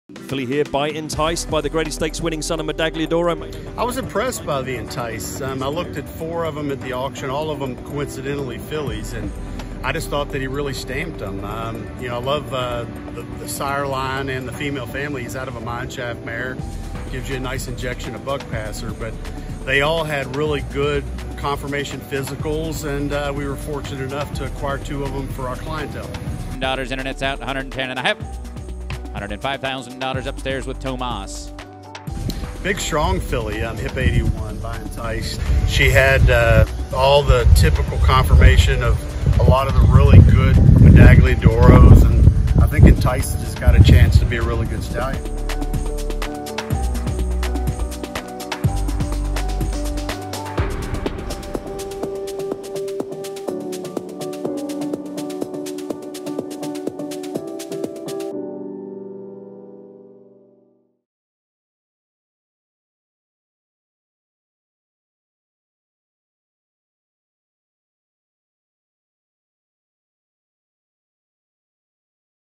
Kenny McPeek on $105k Enticed yearling at Fasig July
Hear what trainer Kenny McPeek had to say about purchasing two yearlings by Enticed at Fasig-Tipton July, including a filly for $105,000!